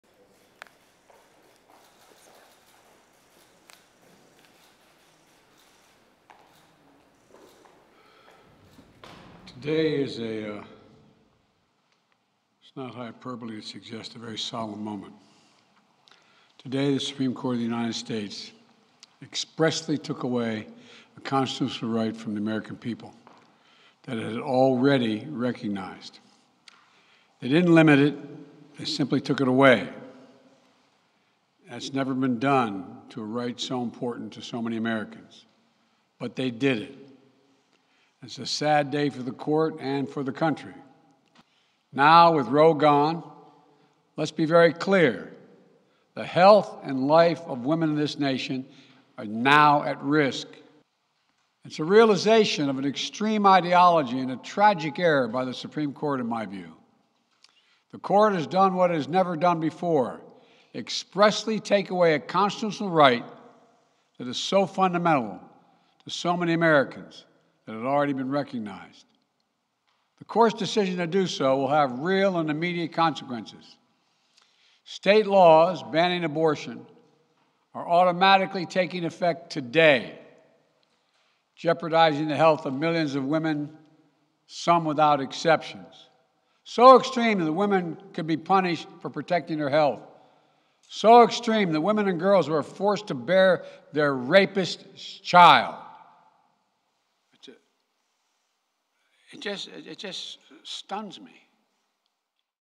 Biden calls abortion ruling 'a sad day' for country
President Joe Biden says "it's a sad day for the court and the country" after the Supreme Court overturns Roe v. Wade, the decision that legalized U.S. abortion. "The health and life of women across this nation are now at risk," he says. (June 24)